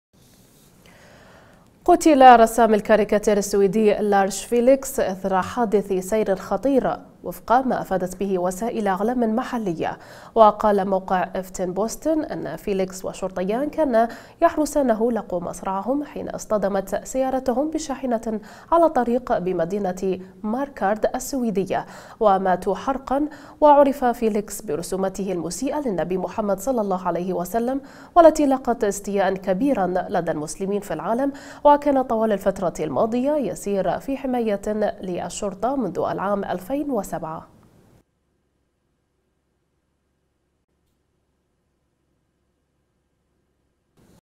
مقتل رسام كاريكاتير اشتهر برسوماته المسيئة للنبي محمد إثر حادث سير مروع
قُتل رسام الكاريكاتير السويدي "لارش فيلكس" إثر حادث سير خطير، وفق ما أفادت به وسائل إعلام محلية. وقال موقع افتن بوستن إن فيلكس وشرطيان كانا يحرسانه لقوا مصرعهم حين اصطدمت سيارتهم بشاحنة على طريق بمدينة ماركارد السويدية.وماتوا حرقا..وعُرف "فيلكس" برسوماته المسيئة للنبي محمد"صلى الله عليه وسلم " والتي لاقت استياءً كبيرا لدى المسلمين في العالم وكان طوال الفترة الماضية يسير في حماية الشرطة منذ العام 2007.